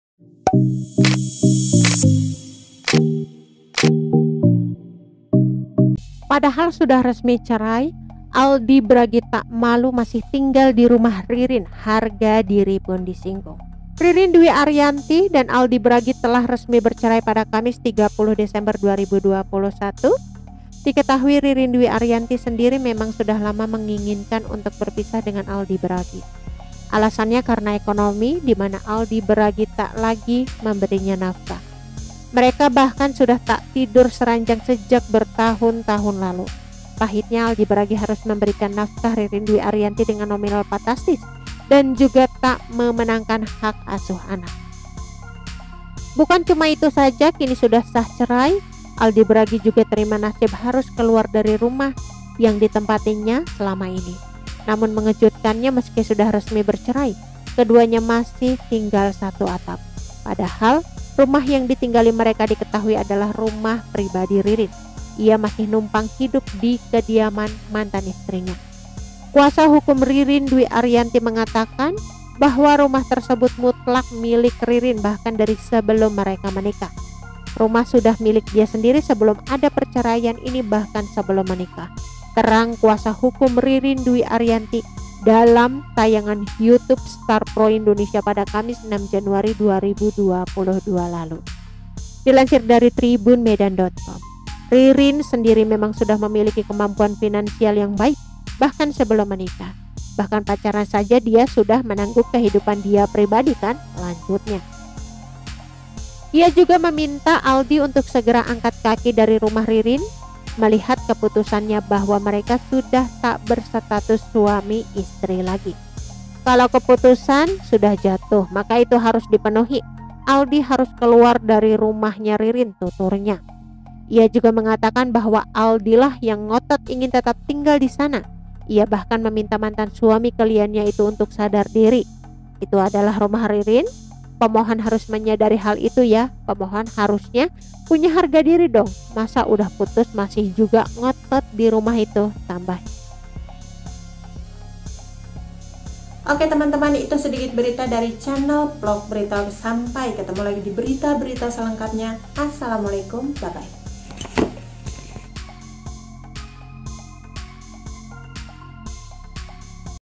Harga Diri Aldi Bragi Disentil, Duda Ririn Dwi Ariyanti Masih Betah Numpang di Rumah Mantan Istri
Ririn Dwi Ariyanti dan Aldi Bragi telah resmi bercerai pada Kamis (30/12/2021).

Diketahui, Ririn Dwi Ariyanti sendiri memang sudah lama menginginkan untuk berpisah dengan Aldi Bragi.

Alasannya karena ekonomi, dimana Aldi Bragi tak lagi memberinya nafkah.

Artikel ini telah tayang di Tribun-Medancom